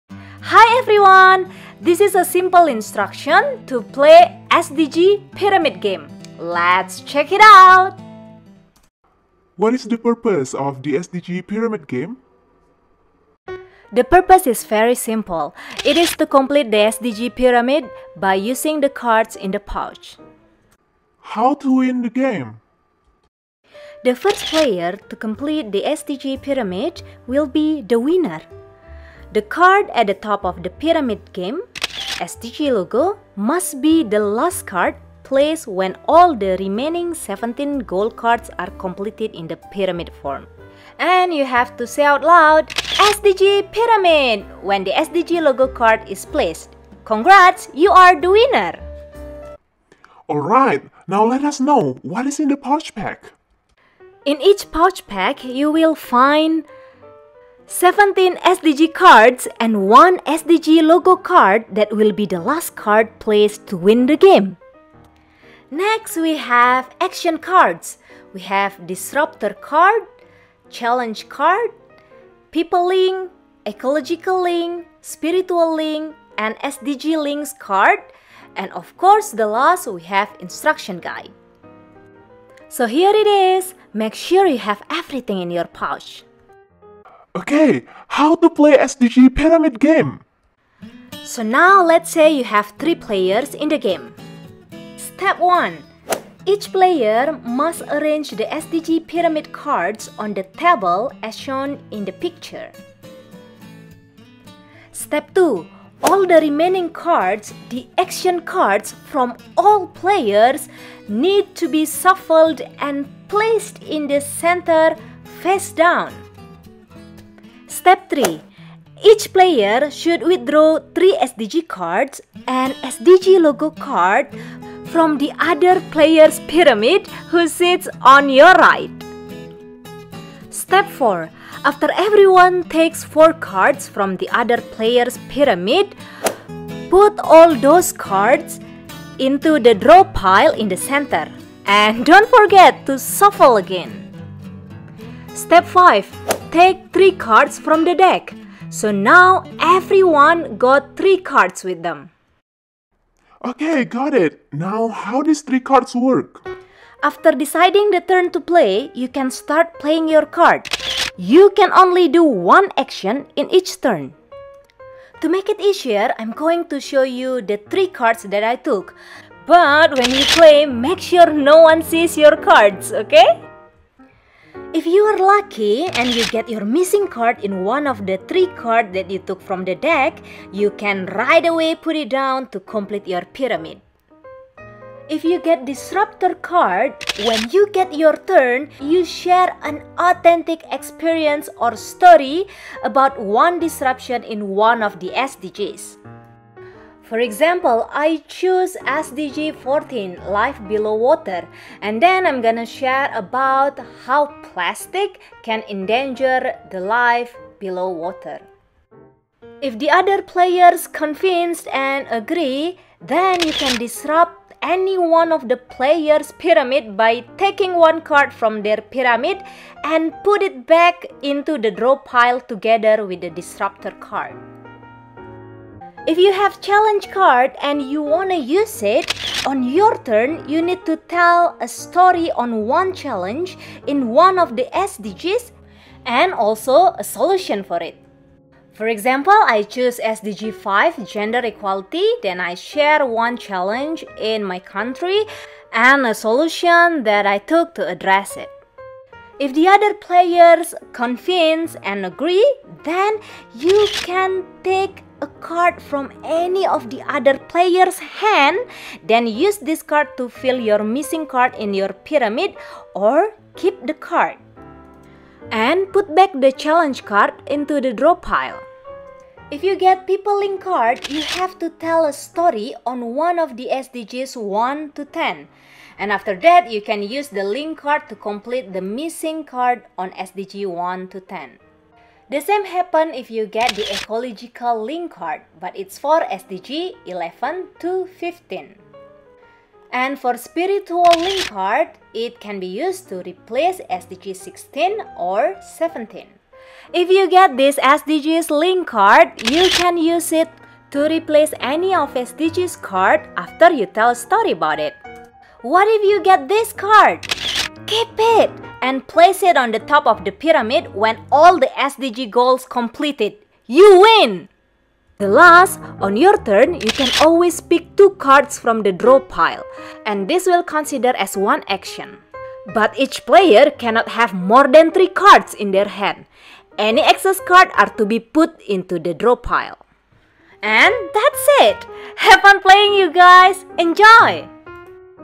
SDG Pyramid Game: How to Play Sustainable Development Goals (SDG) Pyramid Game
In this video I am telling you how to play SDG Pyramid Game. SDG Pyramid Game is created with the intention to create and promote awareness of SDGs and to internalize these goals to the world through the three harmonies. Curious to know more?